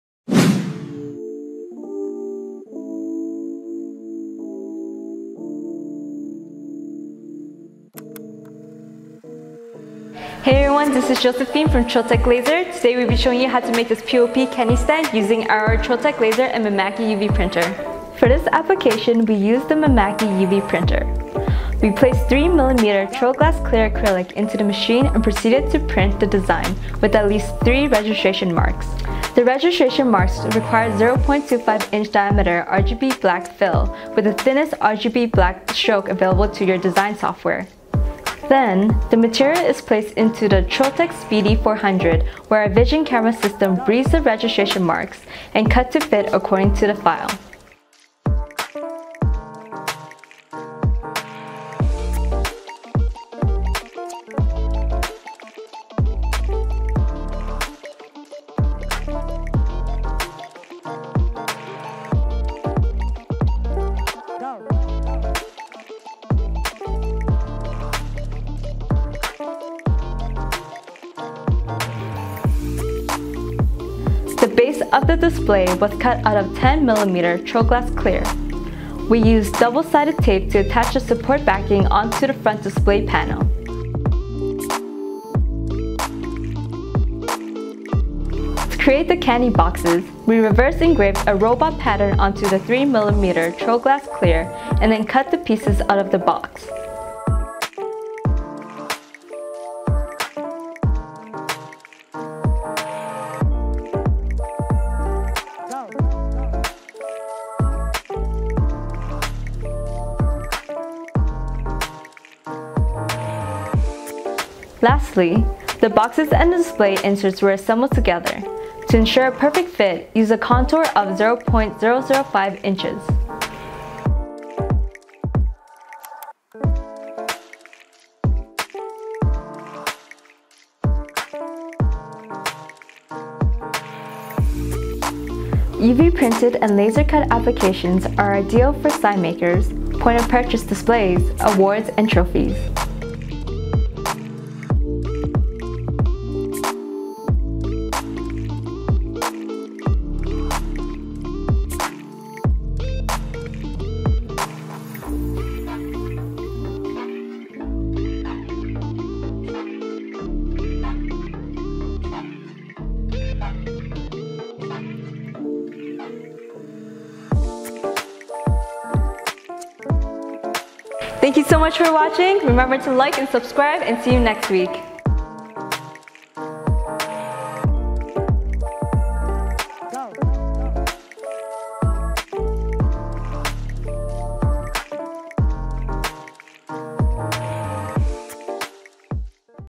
Laser Cut and UV Printed Promotional Display | Trotec Laser
Join us as we dive into the world of laser cutting and UV printing to create an extraordinary Promotional Display using a Mimaki UV printer and our Trotec Speedy400 laser cutter.

In this video, we take you through the entire process step by step. Watch as we UV print the background, complete with 0.25" registration marks for precision. Utilizing our advanced Trotec Vision camera system, we seamlessly ensure that the acrylic is cut precisely along the intended lines, as outlined in the artwork file.

By laser engraving select components of the POP display, we add an extra layer of detail and sophistication. Finally, witness the assembly process as we bring all the elements together to craft a visually stunning and functional display piece.

💥 LASER SETTINGS 💥
Laser: Trotec Speedy 400 100w
Engrave: 55 power, 100 speed
Cut: 100 power, 0.8 speed